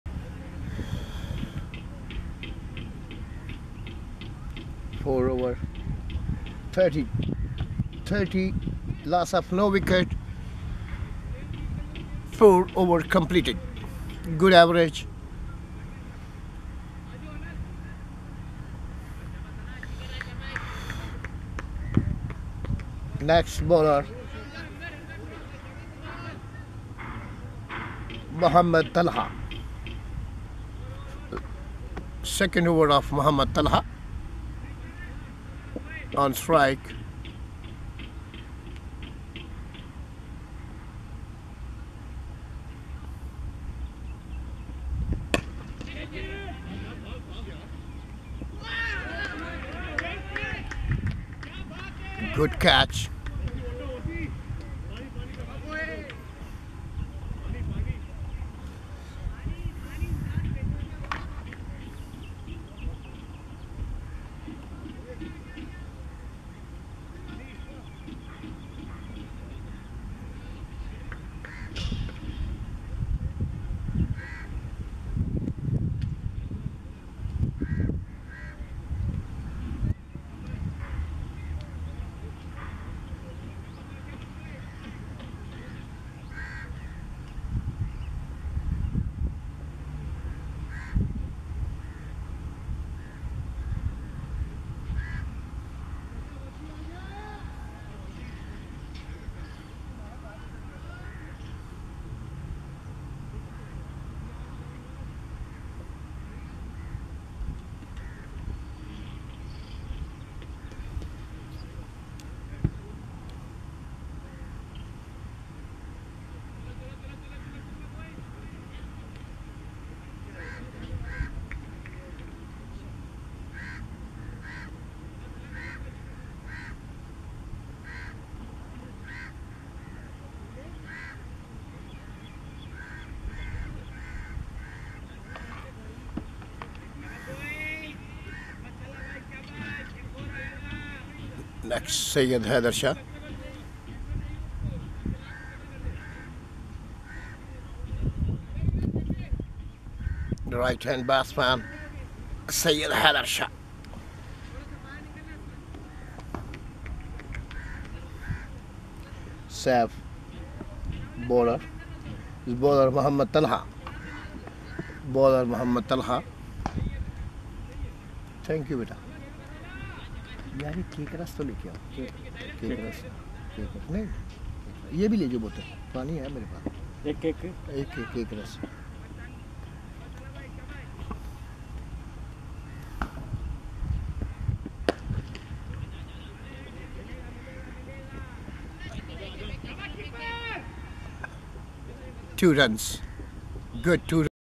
MATCH990 P=8 (SYED HAIDER SHAH 253rd 50's R=69 B=40(NOT OUT) TOTAL RUNS 37,963 (Z GAMES STRIKERS WON
DUBAI TOUR HAWKS CRICKET ACADEMY HYDERABAD PAKISTAN